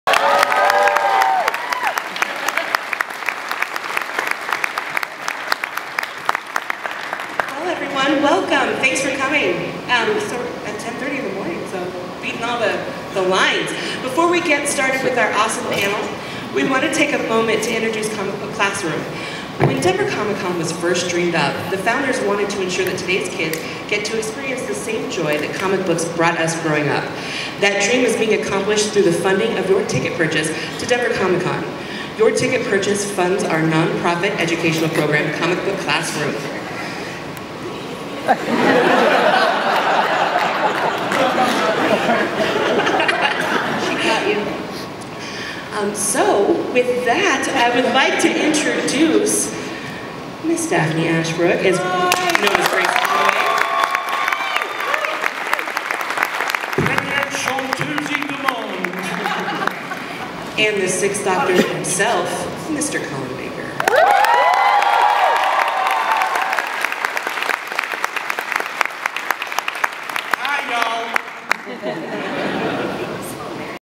Colin Baker and Daphne Ashbrook Panel Opening: Denver Comic Con 2013
Beginning of the first Denver Comic Con 2013 Panel with Colin Baker and Daphne Ashbrook.  Also joining them onstage is Heather Maloney (The Geek Port Podcast and Michelle Talkington (Girls of Geek 12 and Denver Comic Con TV). Heather is mocked by The Doctor, how awesome is that?